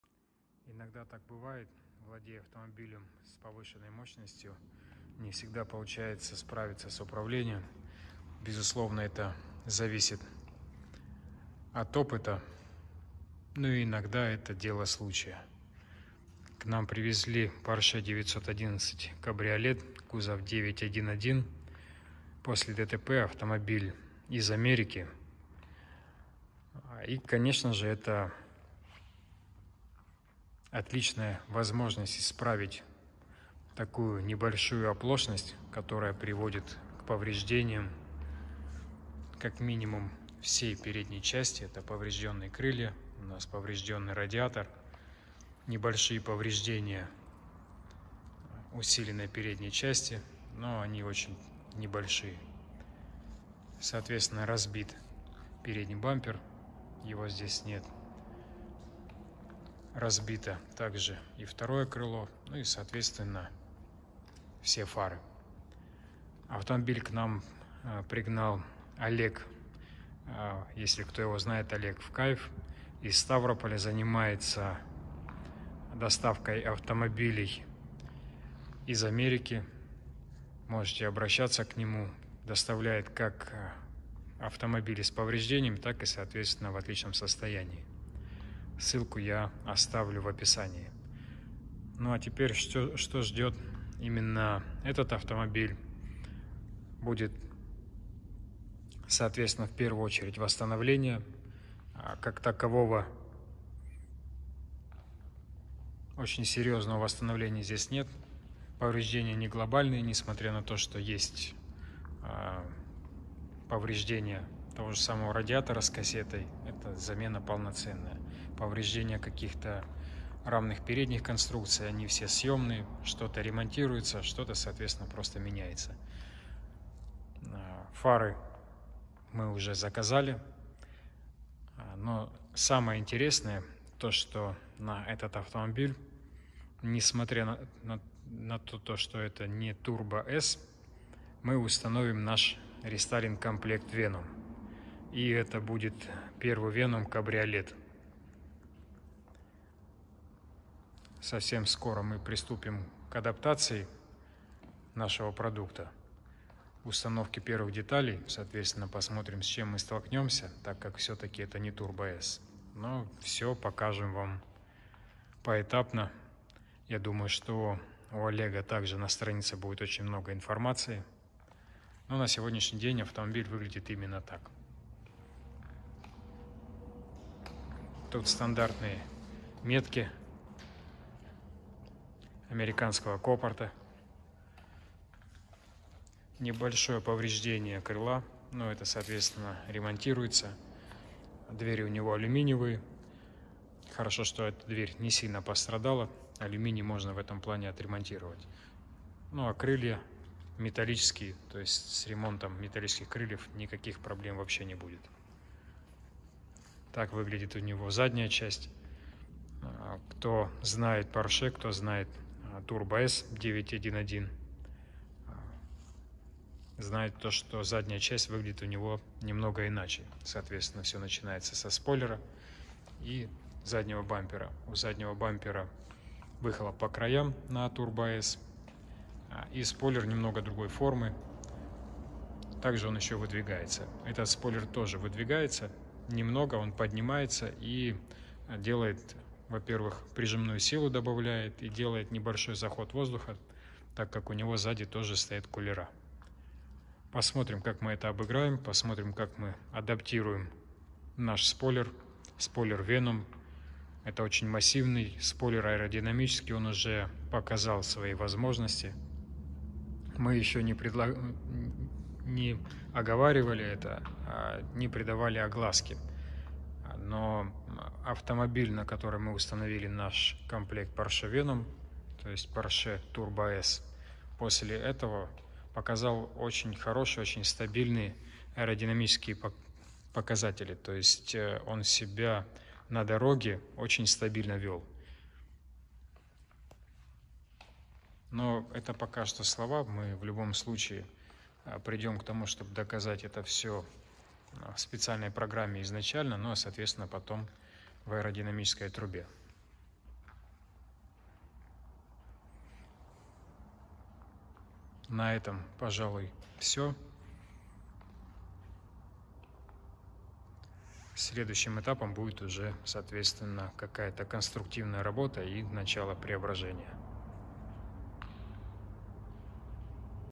Porsche 911 991 Кабриолет + обвес с расширением Porsche VIRUS скоро
Сегодня решили выложить для вас видео с проектом Porsche VIRUS кабриолет на стадии работ, мы понимаем что вам интересны не только авто до рестайлинга и результат, но и сам процесс. Ведь зачастую процесс даже намного интереснее результата, как по-вашему?

Наша компания занимается разработкой и производством комплектов рестайлинга для автомобилей премиум-класса.